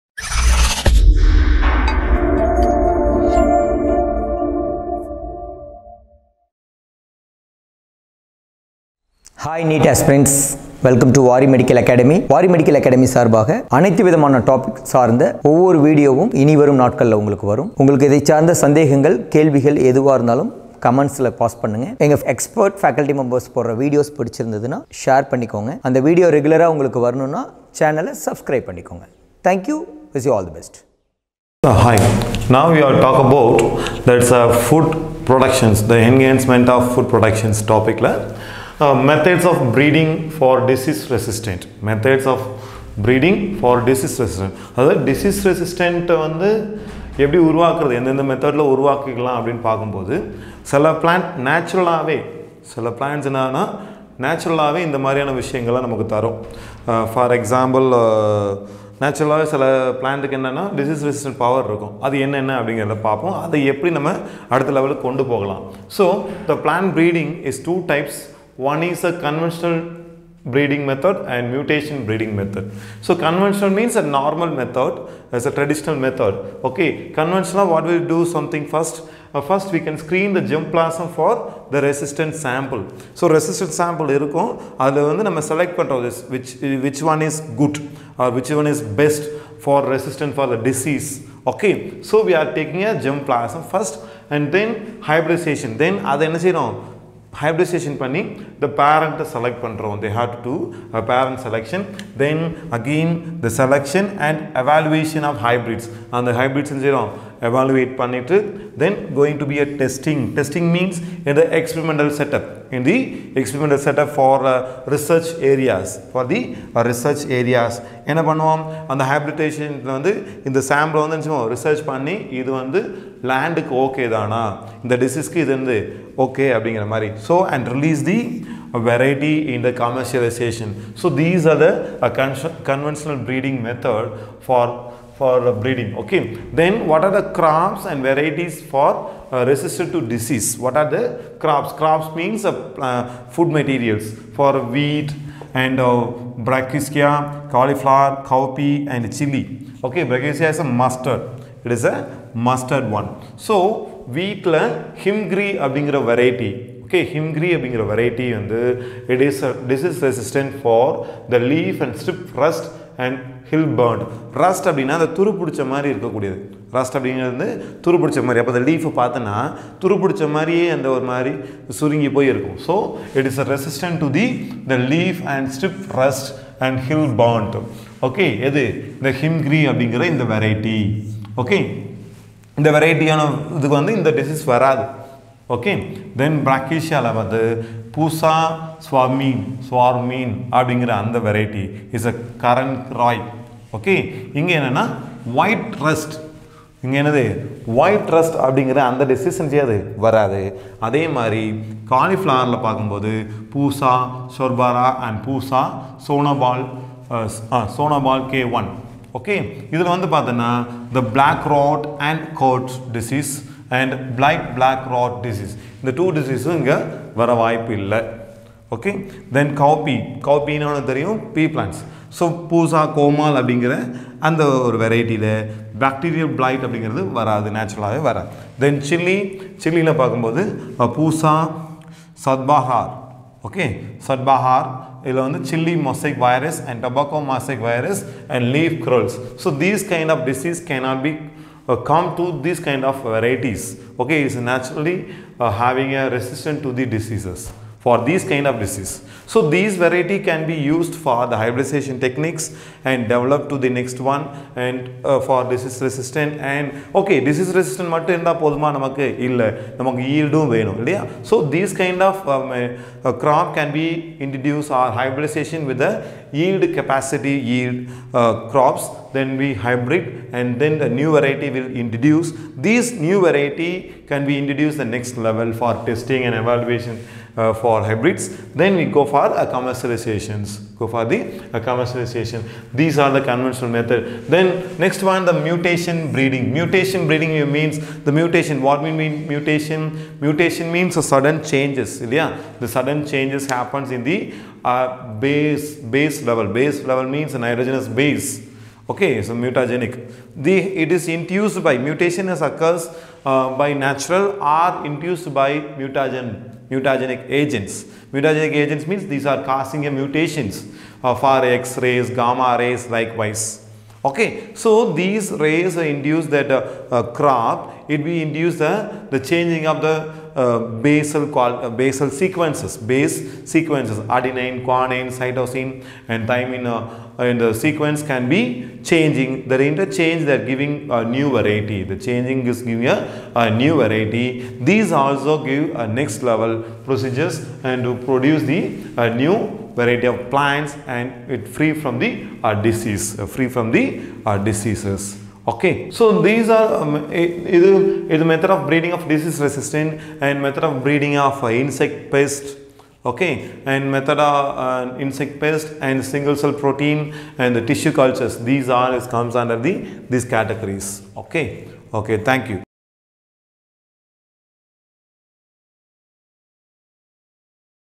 Methods of Breeding for Disease Resistance | Zoology | NEET | NEET 2023 | NEET Syllabus | NEET Exam
Vari Medical Academy Faculty Petchi Muthu Explains Methods of Breeding for Disease Resistance in Zoology for NEET Aspirants.

Vari medical academy helps the students to prepare for the NEET exam, get high marks and provides free classes and study materials.